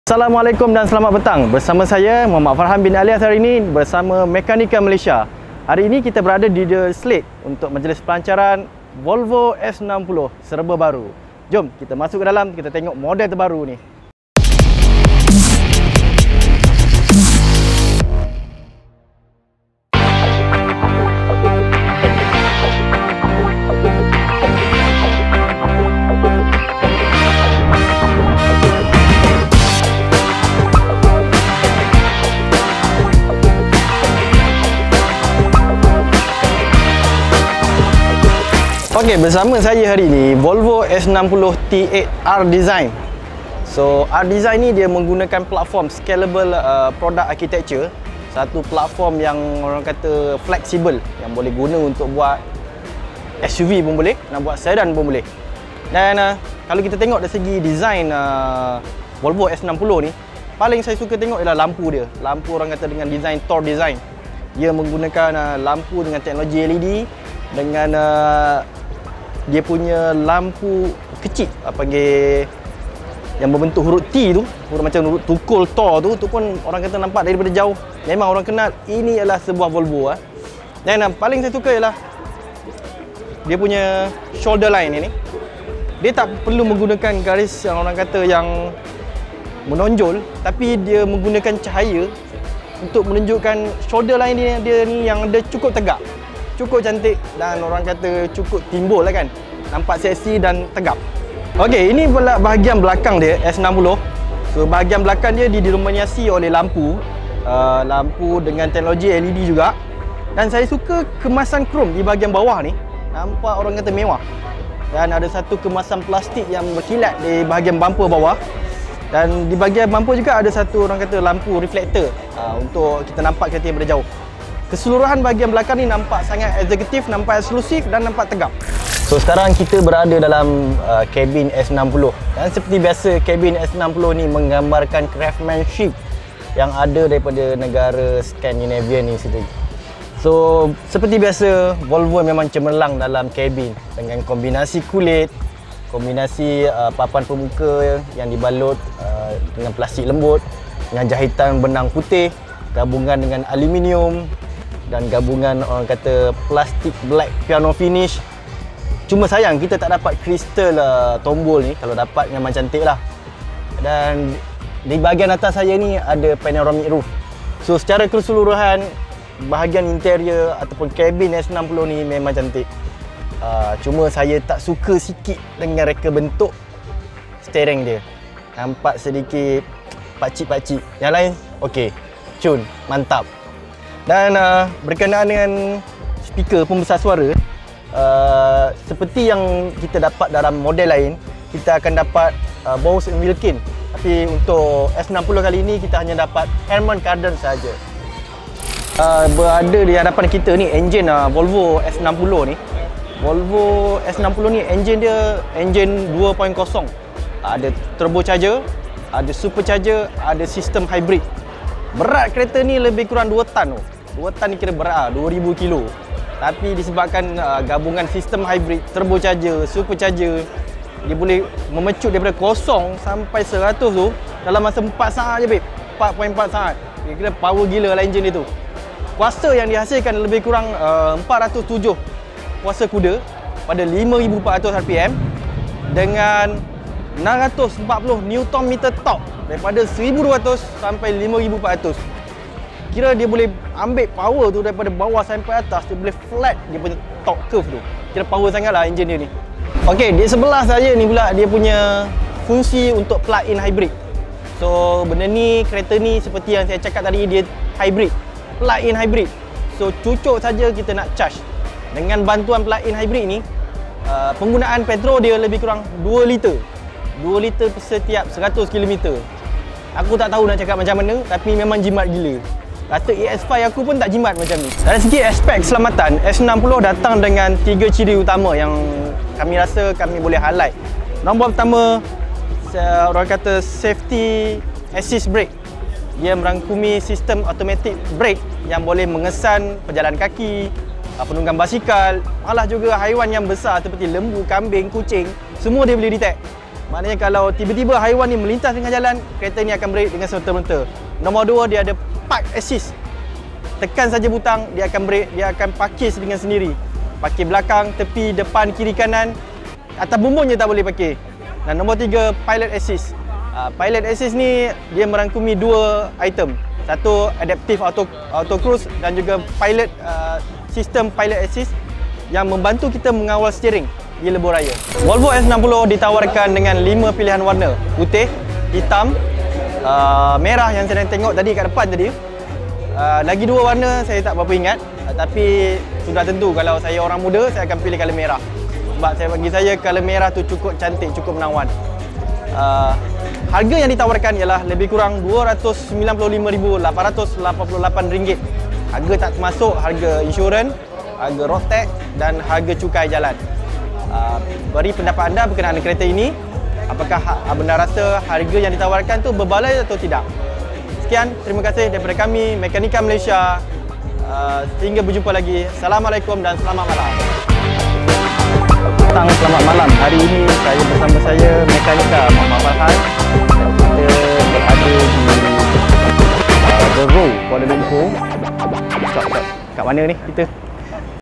Volvo S60 2019 berat 2 tan tapi laju! Apa rahsianya?
Pelancaran Volvo S60 2019 serba baharu.

Orang kata bila kereta berat kereta akan lambat. Tapi Volvo S60 ni tetap laju? Kenapa ya?

Follow Mekanika Malaysia